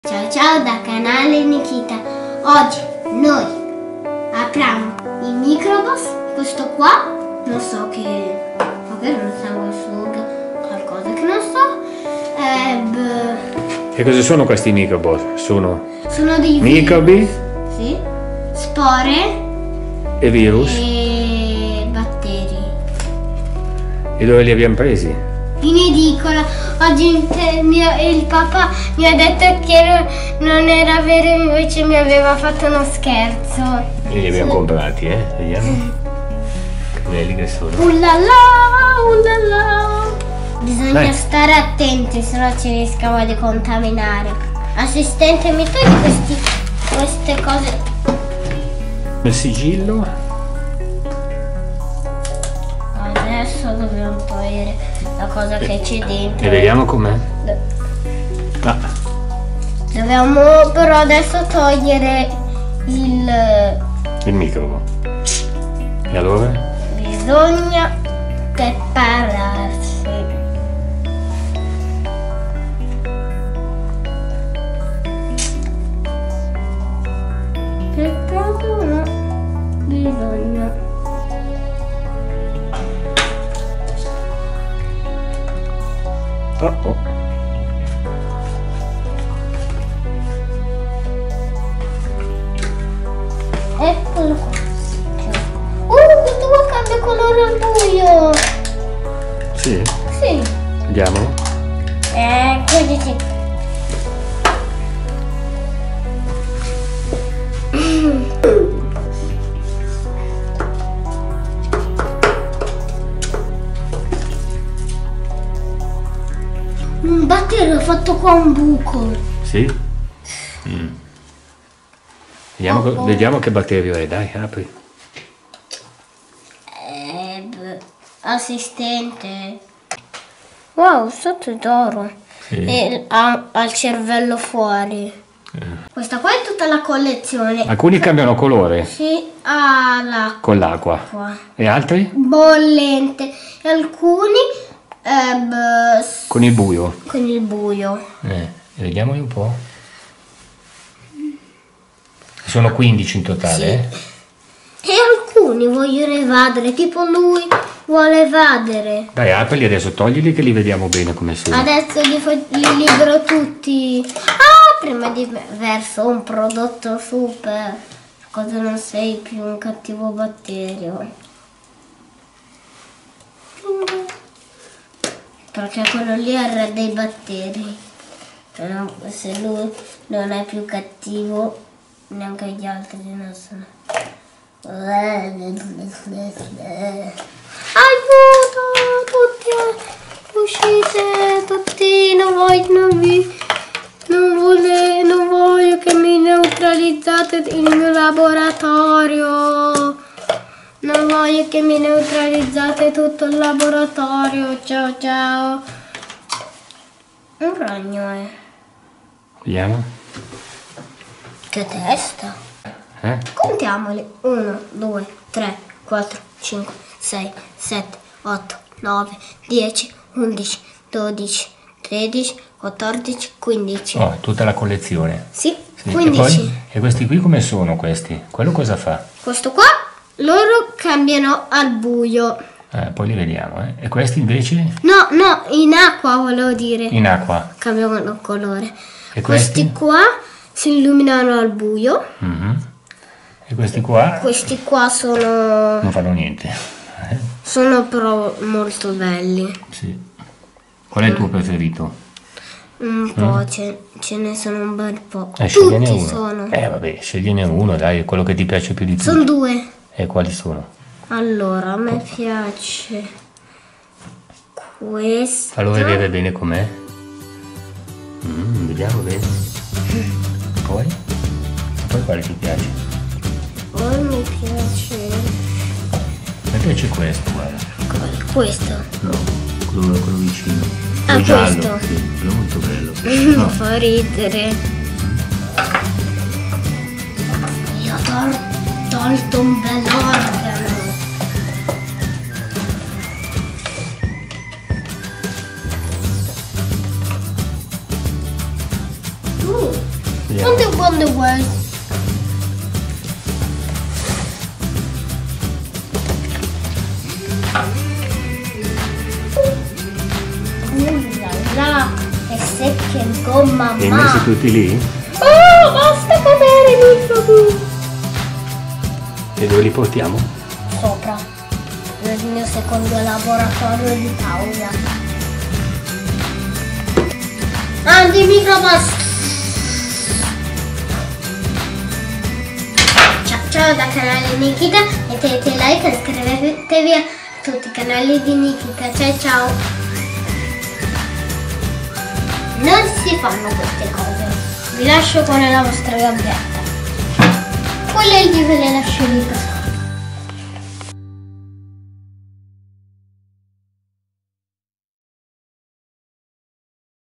Apertura MICROBOSS Giochi Contagiosi Edicola by Canale Nikita
I MICROBOSS sono 15 divertenti personaggi nelle forme di VIRUS, SPORE e  BATTERI
Dai creatori di "The Epic Animals", una nuova collezione contagiosa!! non sono microbi, sono Microboss!    Micro Boss
Trovi le provette in edicola a 2,99 cad. distribuite da Diramix
Ciao Ciao Da Nikita

Коллекция микробов Microboss. Веселые игрушки , полная коллекция микробов . Распаковка на канале Никита смешных симпатичных микробов.